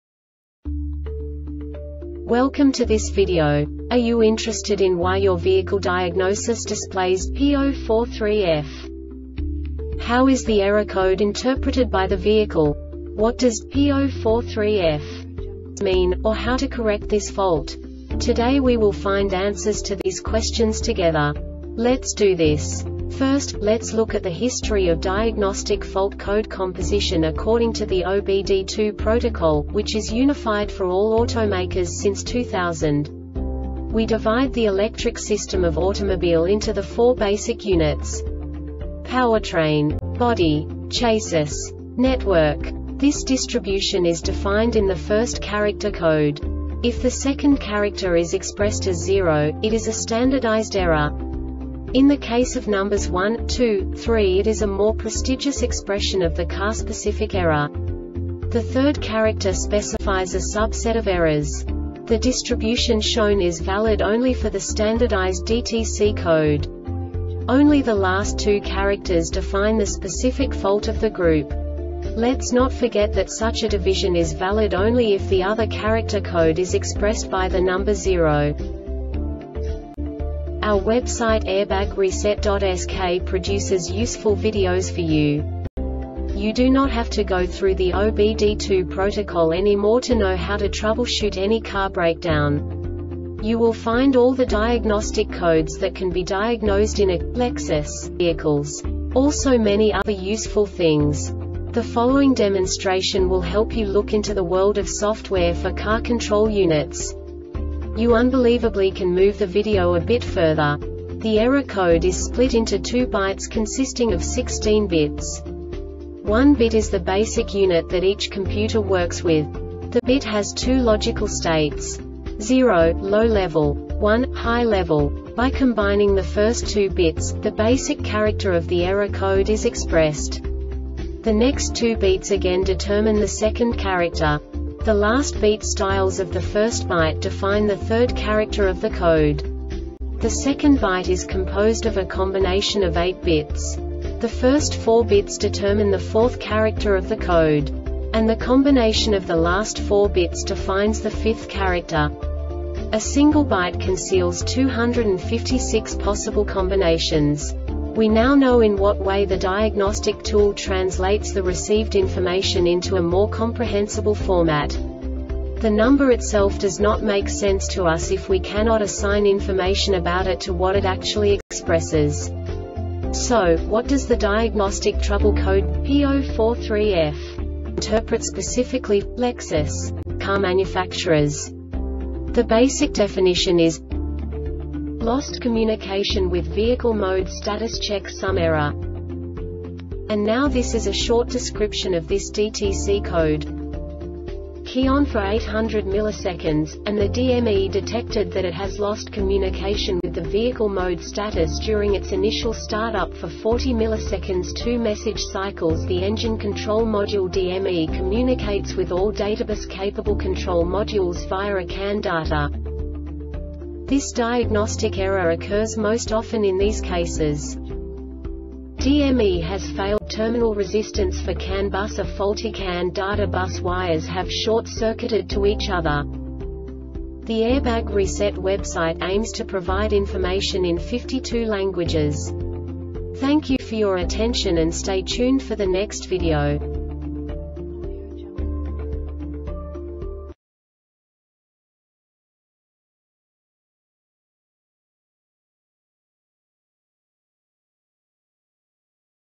DTC Lexus P043F Short Explanation
Contents:
0:21 Basic DTC analysis according to OBD2 protocol standard.
1:48 Insight into programming
2:58 A diagnostically understandable description of the Diagnostic Trouble Code
3:05 Basic error definition

Make:
Lexus
Code:
P043F
Definition: